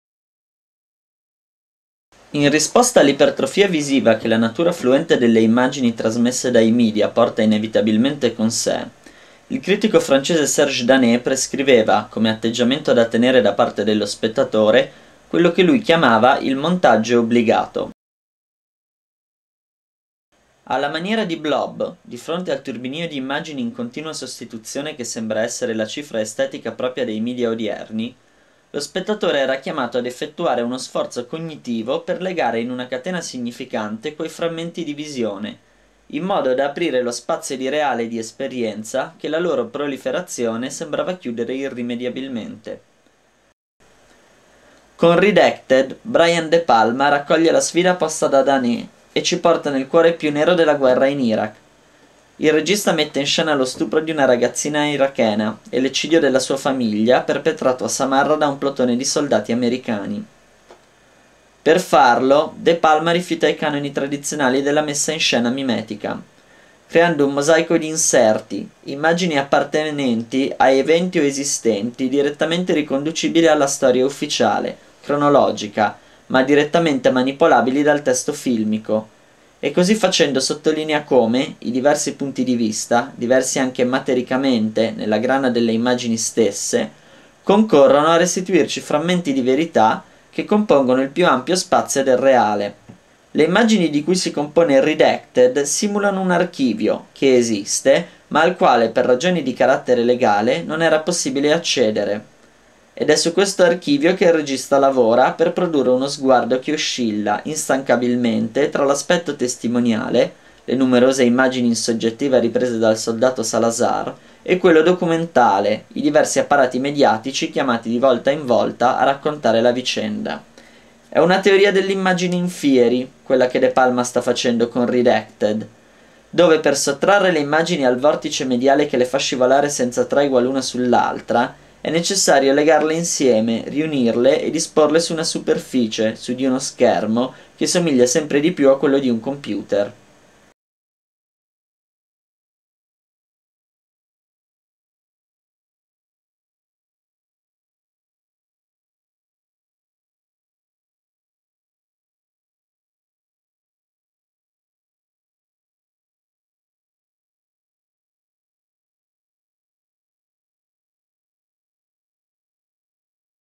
Videocritca di Redacted (Brian De Palma) di Flavio Pintarelli
Redacted è un film di Brian De Palma che racconta dello stupro di una quindicenne irachena e l'eccidio della sua famiglia, perpetrato da alcuni soldati americani. De Palma fa emergere la vicenda dalle immagini di una seria ampia e variegata di dispositivi mediali, accuratamente simulati. Tramite questo archivio di inserti, lo sguardo oscilla tra la dimensione testimoniale e quella documentari dell'immagine, senza essere mai completamente né l'una né l'altra.